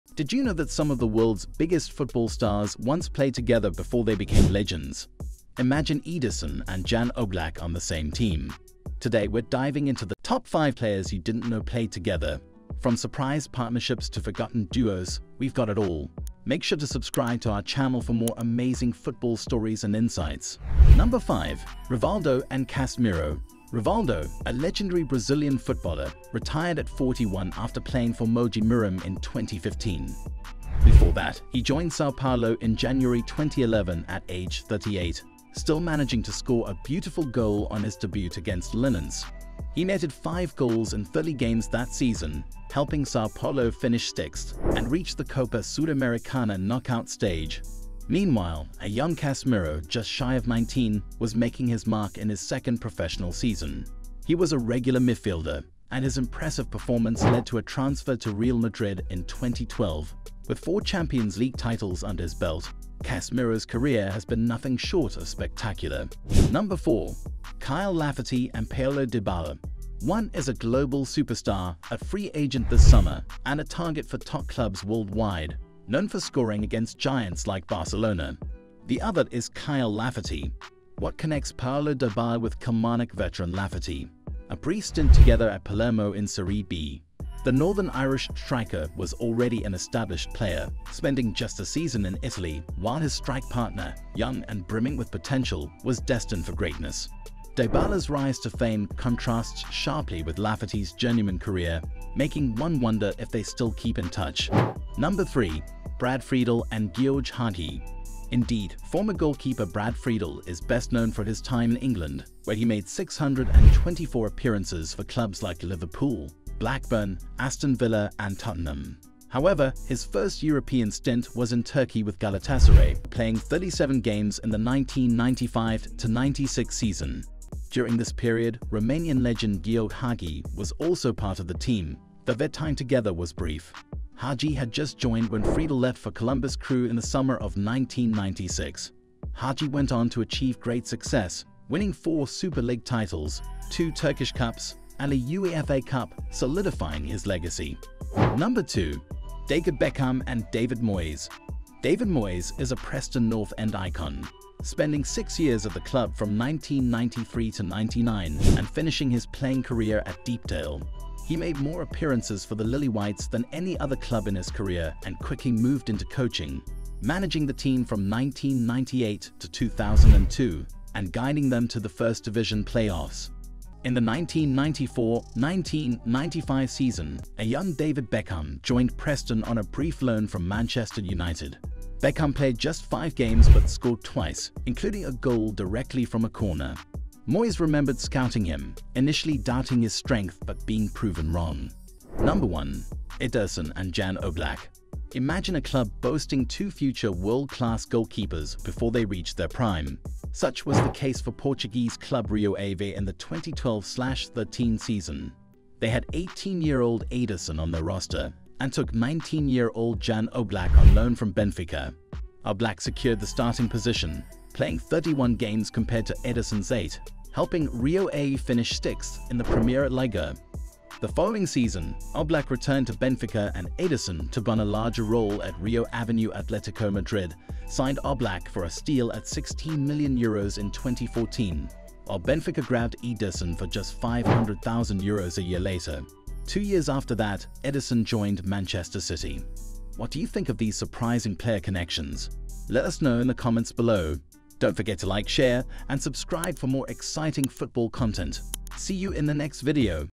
Top Football Duos You Never Knew Played Together!
Get ready to be amazed as we explore the hidden connections in football history, uncovering the top player duos you never knew shared the pitch! From surprising partnerships to brief stints together, these stories will change your perspective on the beautiful game.